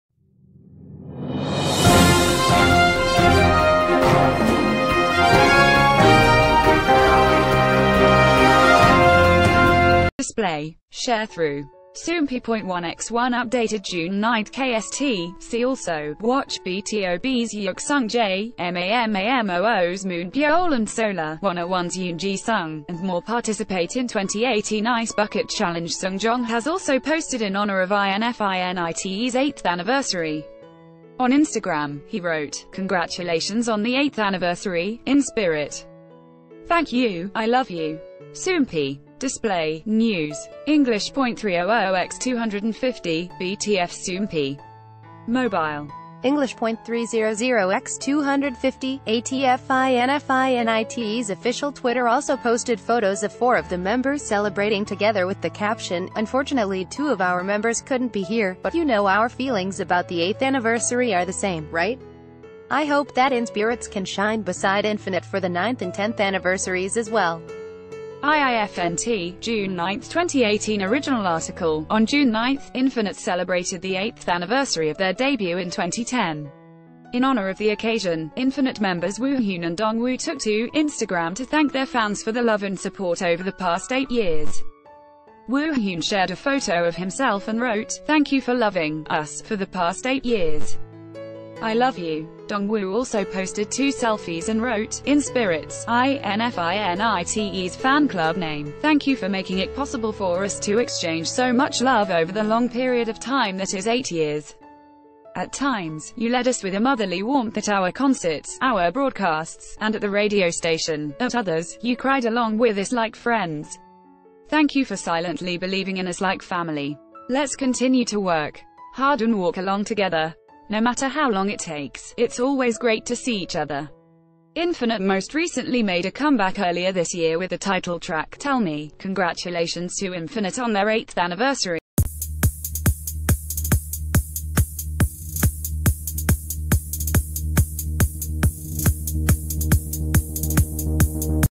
INFINITE's Woohyun And Dongwoo Share Their Thoughts On 8th Anniversary
Display. Sharethrough. Soompi.1x1 Updated June 9 KST:  See Also: Watch: BTOB's Yook Sungjae, MAMAMOO...